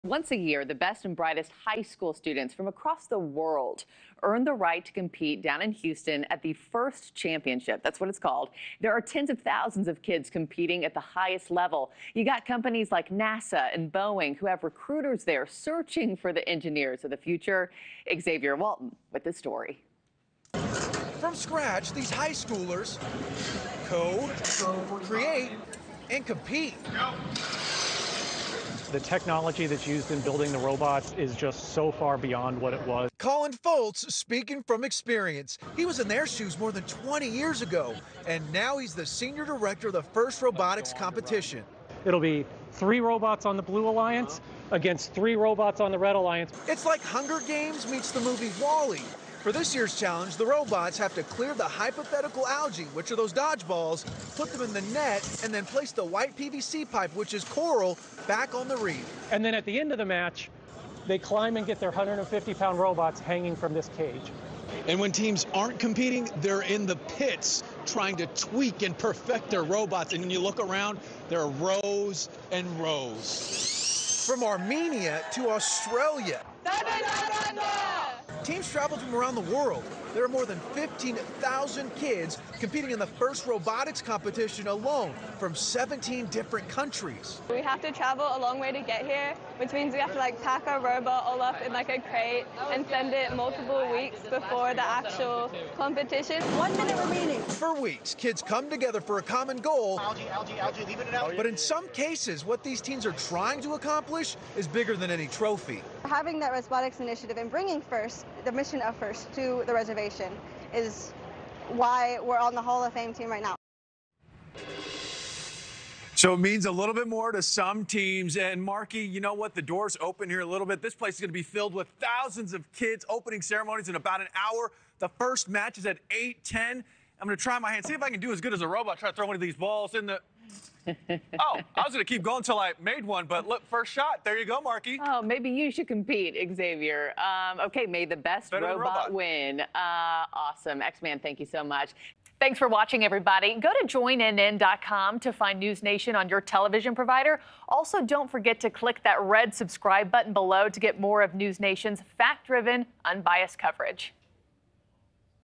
Robot rumble: Inside the FIRST Championship | Morning in America
Tens of thousands of high school students from across the globe are competing at the FIRST Championship, a four-day robotics competition. NewsNation's Xavier Walton is in Houston with the latest.